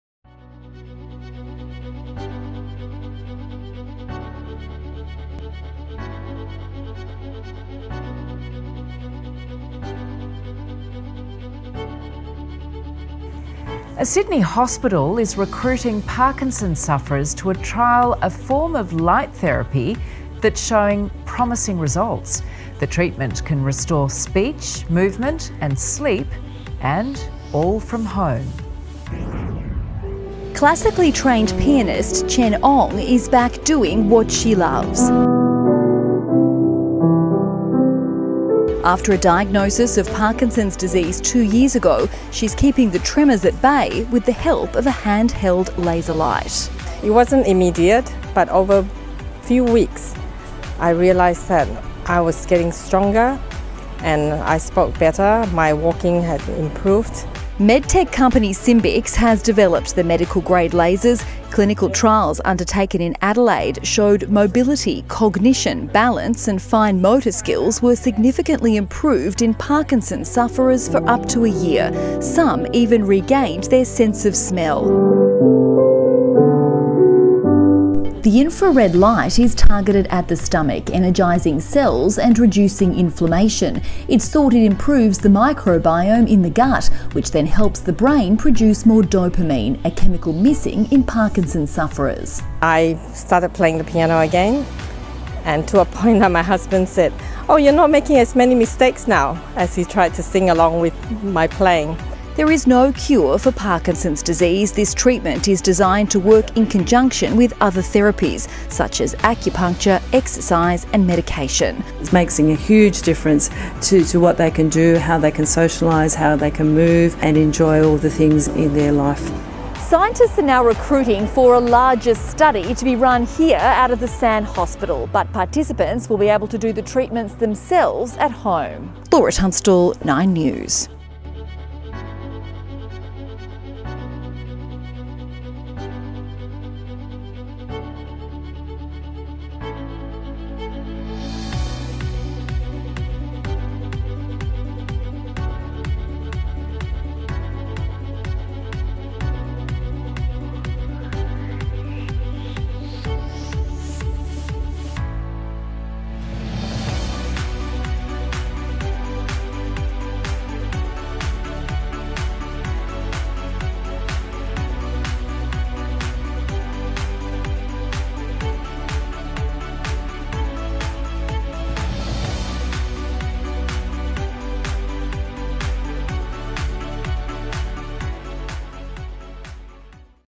栢金遜症患者的新希望!
利用高安全性的低能量激光促使大腦分泌多巴胺，研究指出有助柏金遜症(Parkinson's Disease)患者減少震顫，恢復活動能力，語言能力及甚至嗅覺。